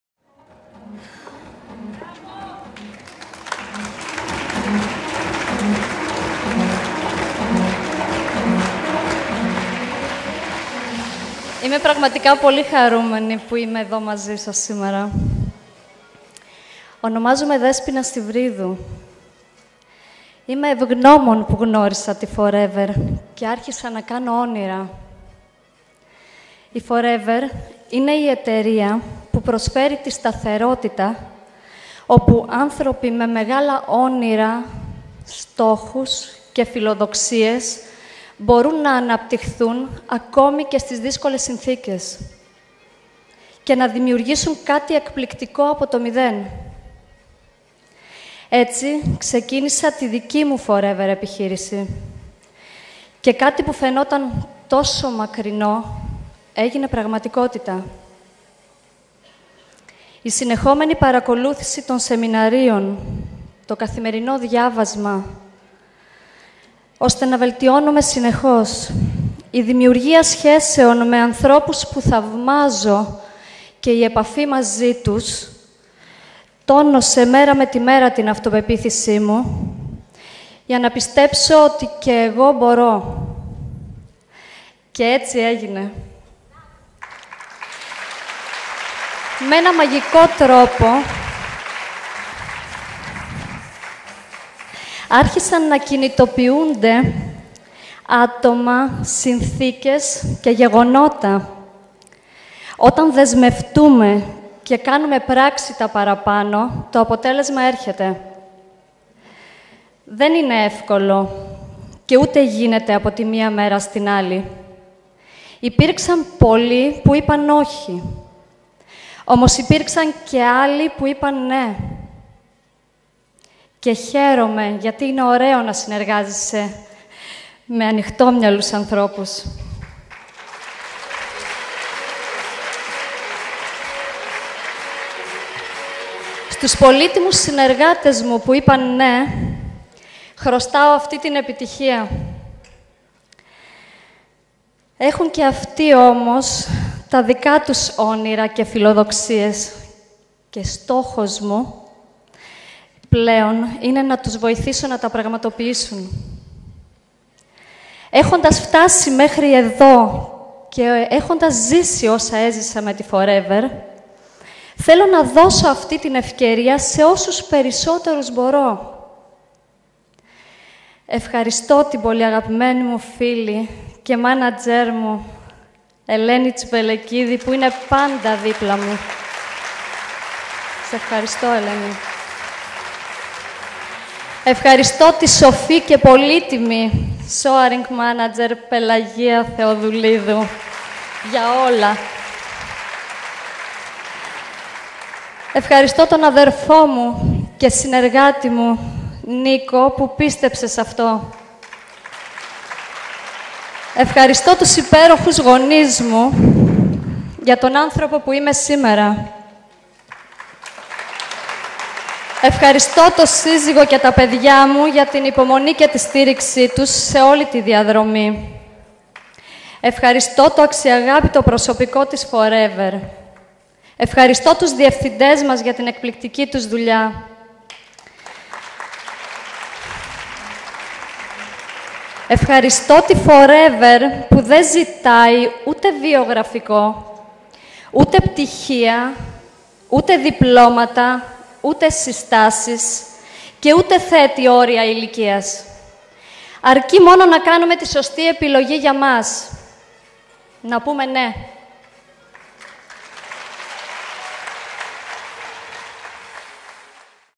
Η νέα manager Συβρίδου Δέσποινα
Η ομιλία της νέας Manager Συβρίδου Δέσποινα στο Success Day Ιουνίου 2015. Συγχαρητήρια!